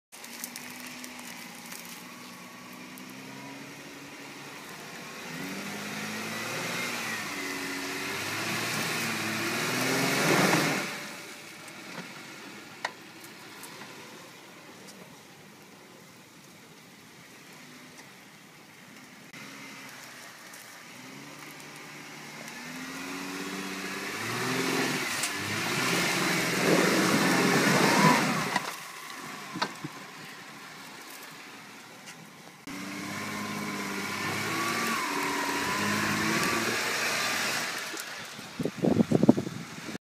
2012 jk
Stock jk climbing a sandy hill